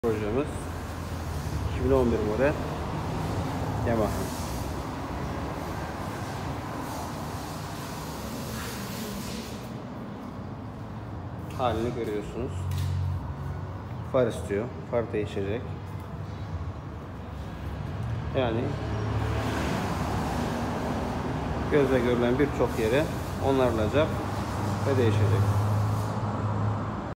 Restorasyon etmek için Yamaha YBR 125 aldım
Motorumuz 2011 model Yamaha ybr125 tir ben bu motoru bir ahırda buldum  ve satın aldım orjinalliğini bozmada ilk günkü gibi toplamaya çalışacam şans dileyin